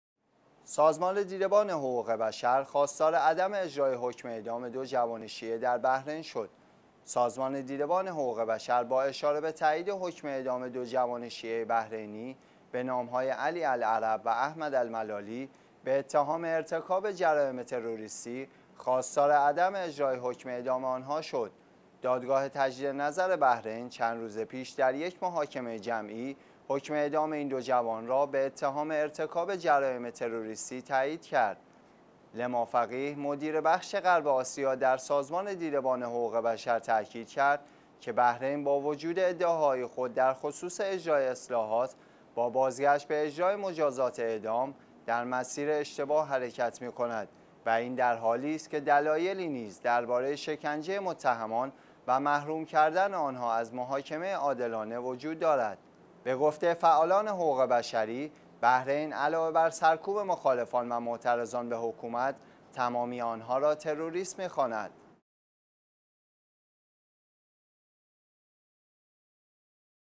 درخواست دیده‌ بان حقوق بشر برای توقف اجرای حکم اعدام ۲ شهروند شیعه بحرینی
سازمان دیده‌ بان حقوق بشر، خواستار عدم اجرای حکم اعدام دو جوان شیعه در بحرین شد.
سازمان دیده‌ بان حقوق بشر با اشاره به تأیید حکم اعدام دو جوان شیعه بحرینی به نام‌های «علی العرب» و «احمد الملالی» به اتهام ارتکاب جرایم تروریستی، خواستار عدم اجرای حکم اعدام آن‌ها شد.
دادگاه تجدیدنظر بحرین چند روز پیش در یک محاکمه جمعی، حکم اعدام این دو جوان را به اتهام ارتکاب جرایم تروریستی تأیید کرد.
«لما فقیه» مدیر بخش غرب آسیا در سازمان دیده‌بان حقوق بشر تأکید کرد که بحرین با وجود ادعاهای خود در خصوص اجرای اصلاحات، با بازگشت به اجرای مجازات اعدام، در مسیر اشتباه حرکت می‌کند و این در حالی است که دلایلی نیز درباره شکنجه متهمان و محروم کردن آن‌ها از محاکمه عادلانه وجود دارد.
به گفته فعالان حقوق بشری، بحرین علاوه بر سرکوب مخالفان و معترضان به حکومت تمامی آنها را تروریست می خواند.